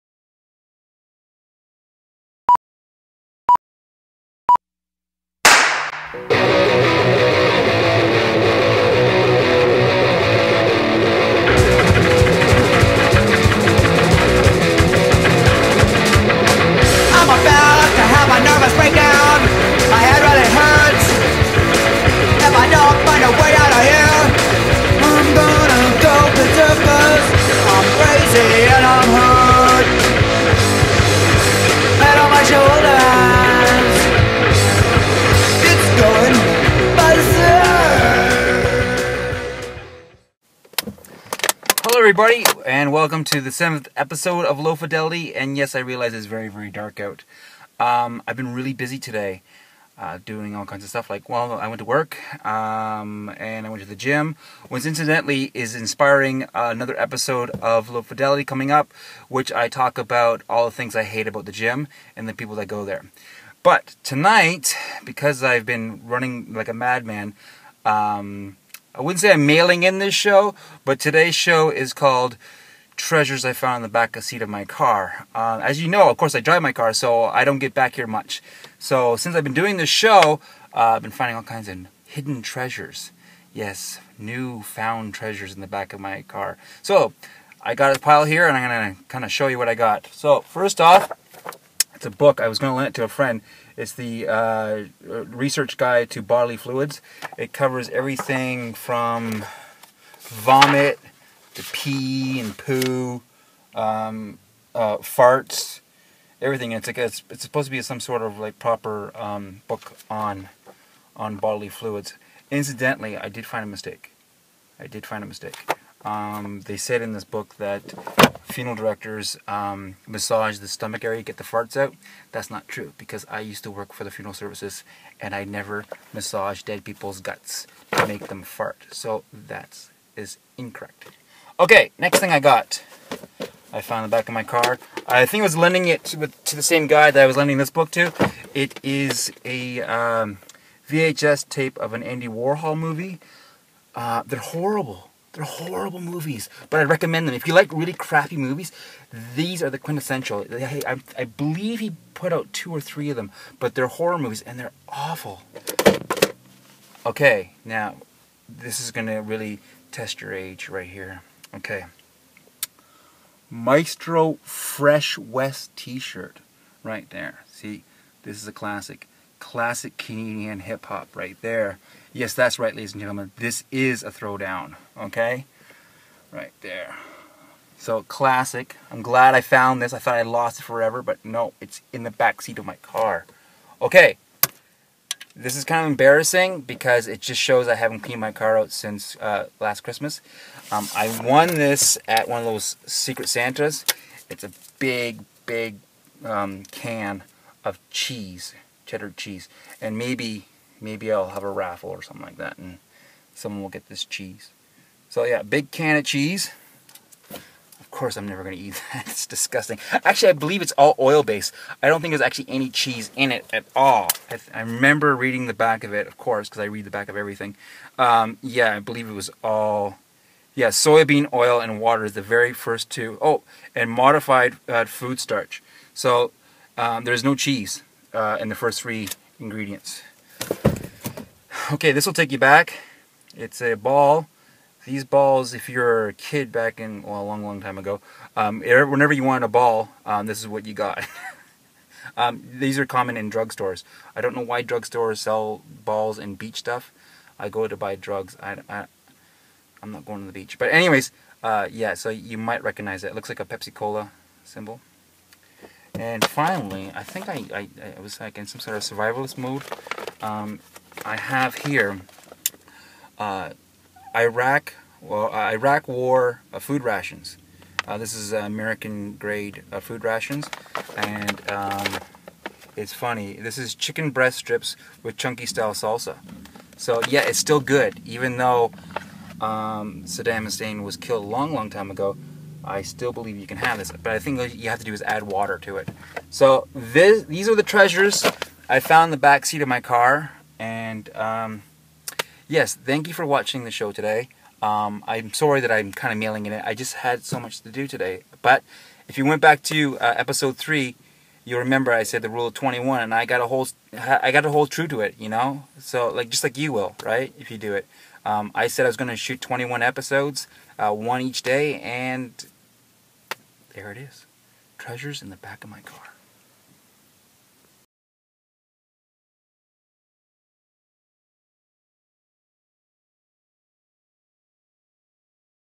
Found Treasure In The Backseat Of My Car
On Low Fidelity, Todd finds some great treasure in the backseat of his car